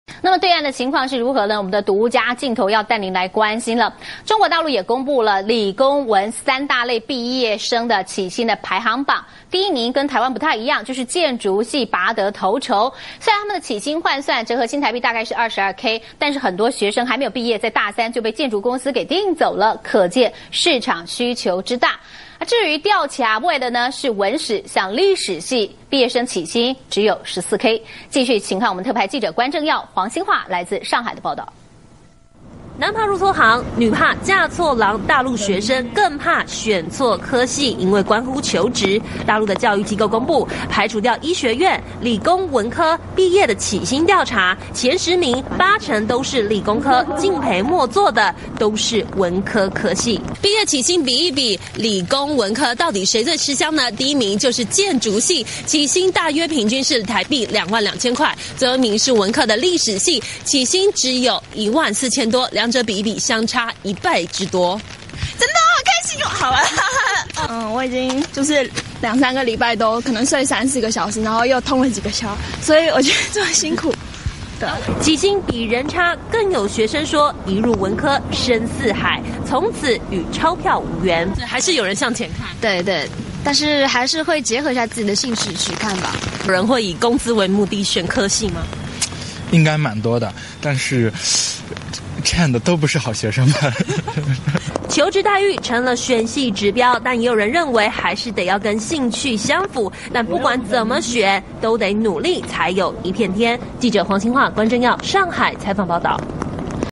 中視新聞》陸畢業生起薪大PK 建築系畢業最吃香
內容轉載自《中時電子報》：
中視新聞》展望未來，年輕人的前途在哪裡？大學的熱門科系，是一個重要的指標！中視新聞獨家企劃，帶您了解兩岸發展新趨勢！先來看到，中國大陸最近公佈的理工文三大類畢業起薪排行榜，第一名是建築系，雖然起薪換算成台幣其實差不多也只有22K，但很多人讀到大三就被建築公司給訂走了，可見市場需求之大！但吊車尾的歷史系起薪就只有14k，請看關鉦耀黃星樺來自上海的報導。